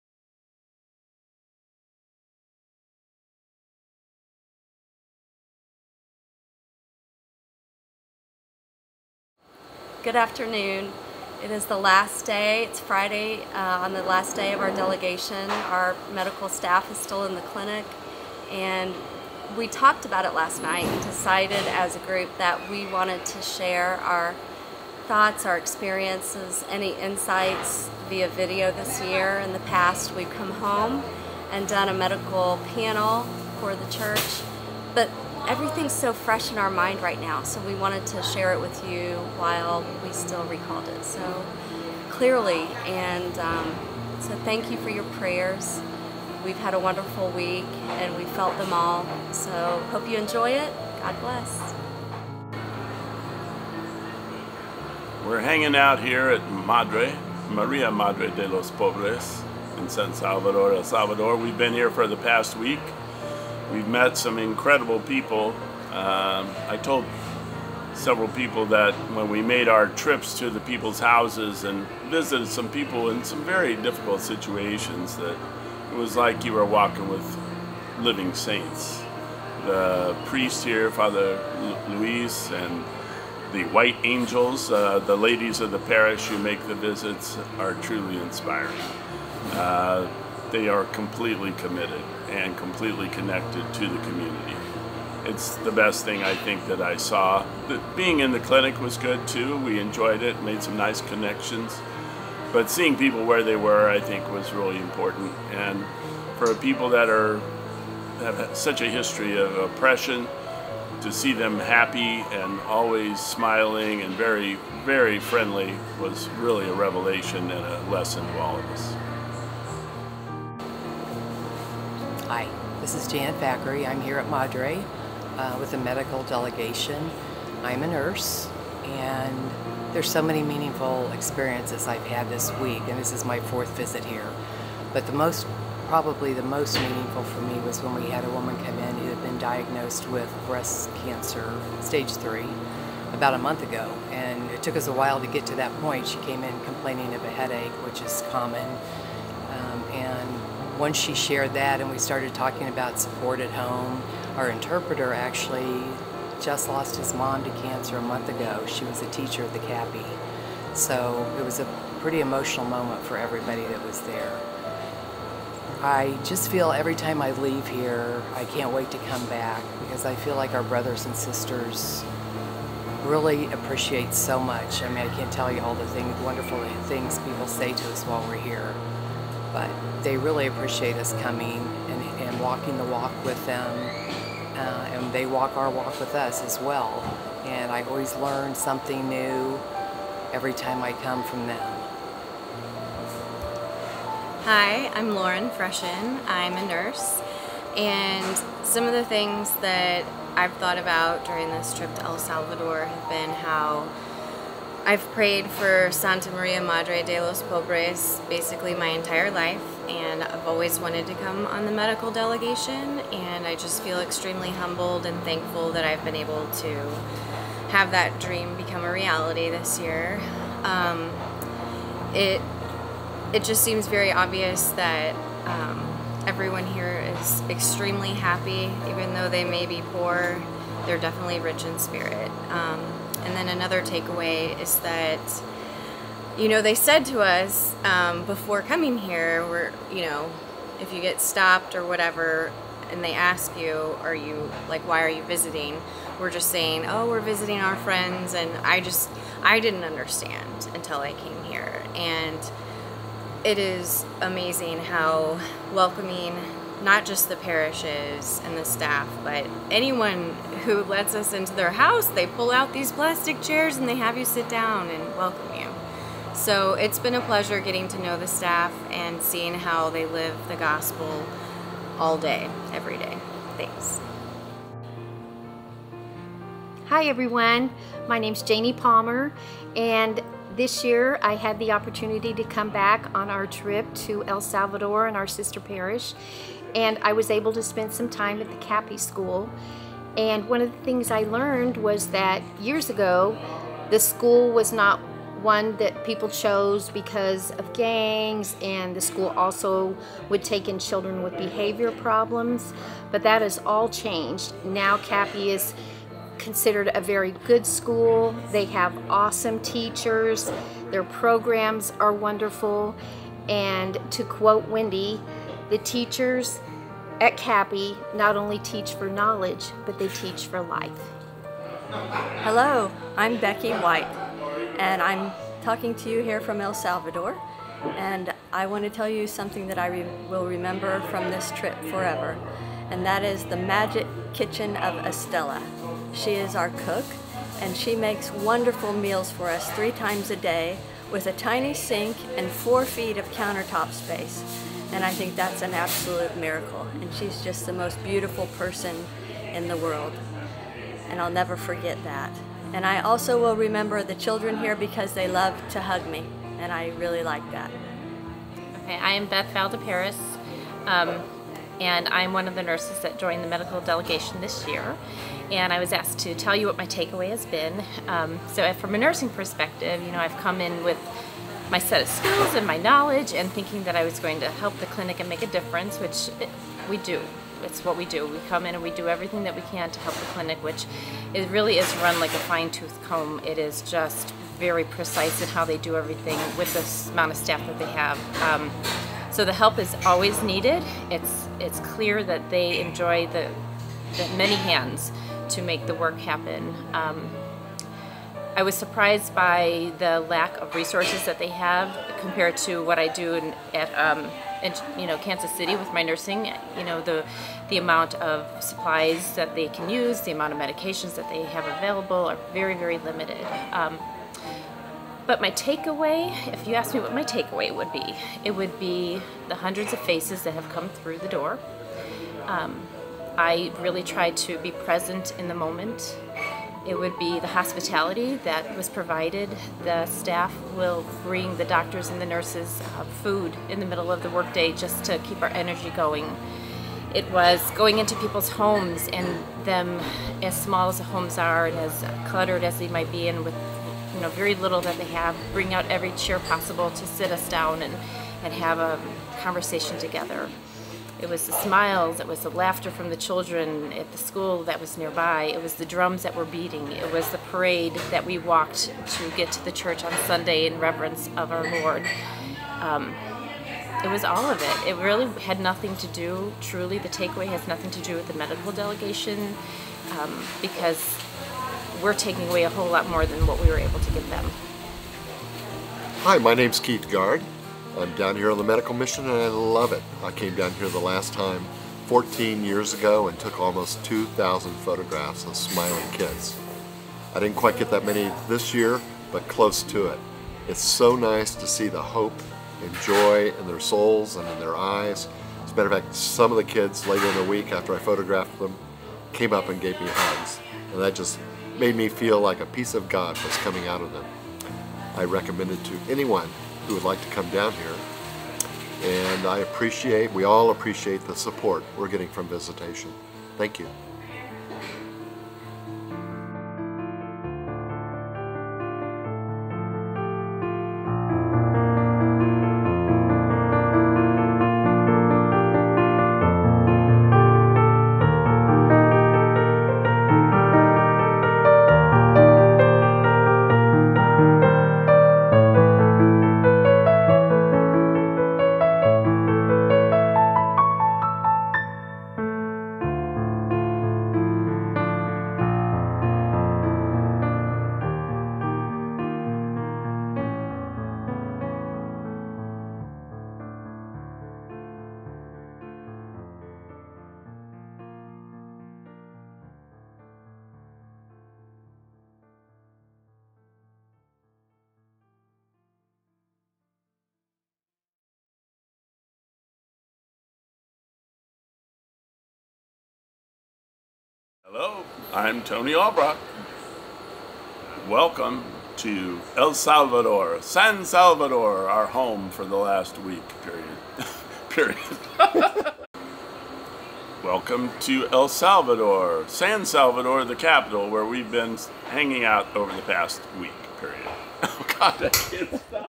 Maria Madre Medical Delegation
Yearly, Visitation Catholic Church in Kansas City, Missouri, sends a medical mission group down to attend to the needs in our sister parish, Maria Madre de los Pobres located in San Salvador, El Salvador. This year it lasted from September 30th to October 7th, 2023. Included in this video are testimonials from the travelers along with photos of our friends at Maria Madre.